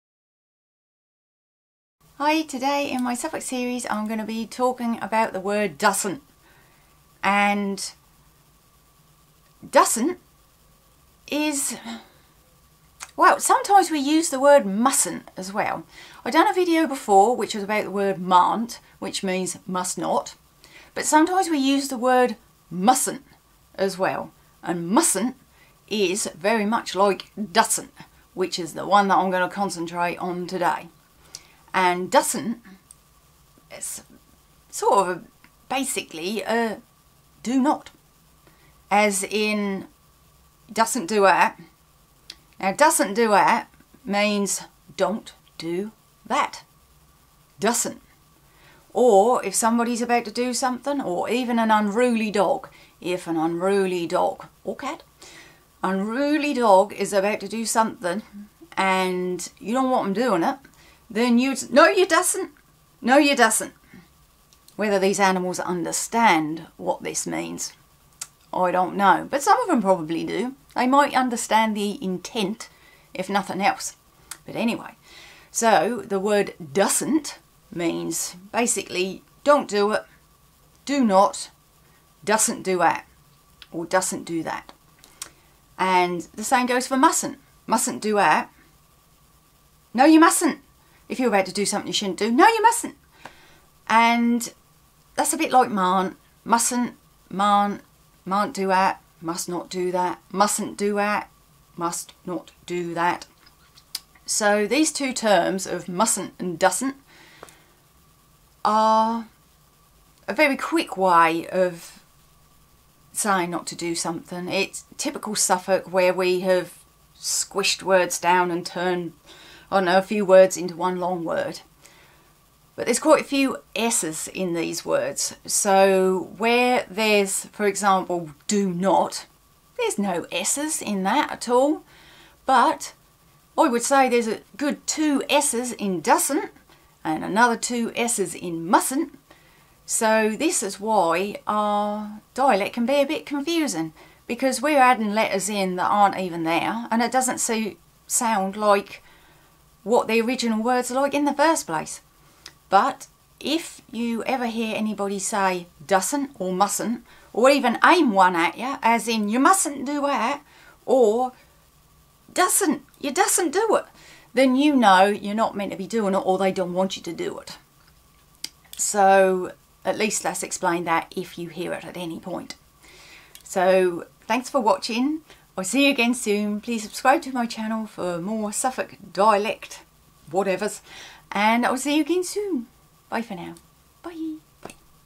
𝐎𝐥𝐝 𝐄𝐧𝐠𝐥𝐢𝐬𝐡 𝐒𝐮𝐟𝐟𝐨𝐥𝐤 𝐚𝐜𝐜𝐞𝐧𝐭 𝐚𝐧𝐝 𝐝𝐢𝐚𝐥𝐞𝐜𝐭, 𝐄𝐚𝐬𝐭 𝐀𝐧𝐠𝐥𝐢𝐚 '𝐃𝐮𝐬𝐬𝐞𝐧𝐭'
In this video for the Suffolk accent and dialect series I'm explaining the term "Dussent". A good old fashioned Suffolk word which basically means "Do not" or "Don't" :)